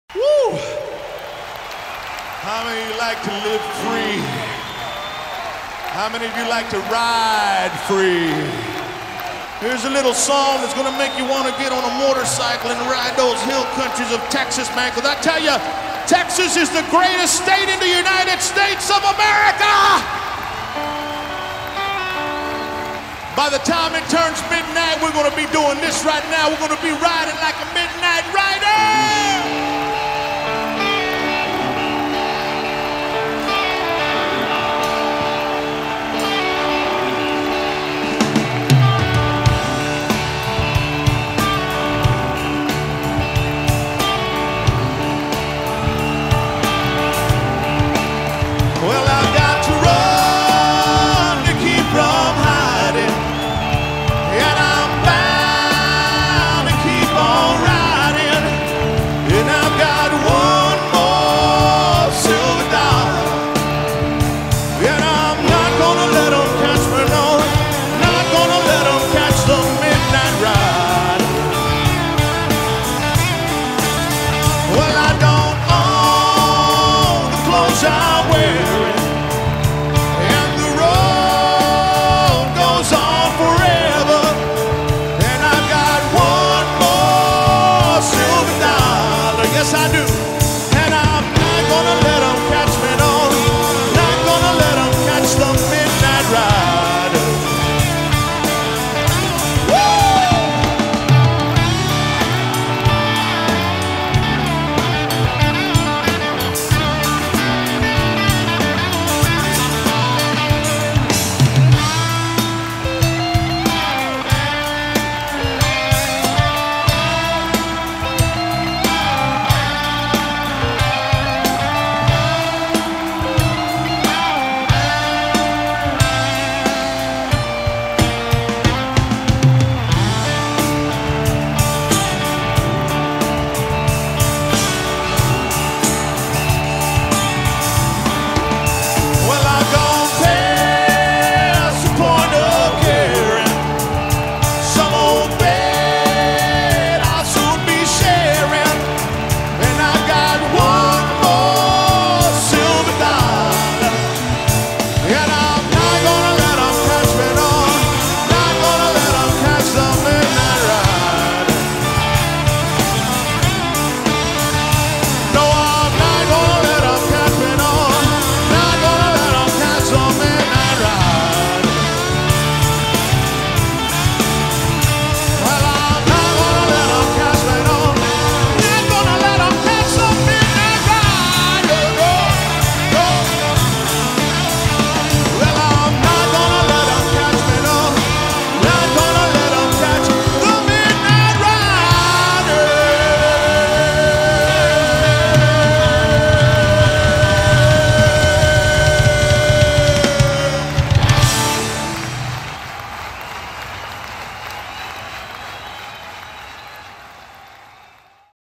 Whiskey D. Live South Padre Island 2019
Whiskey D. traveled to South Padre Island on what turned out to be a chilly Spring Break Concert Event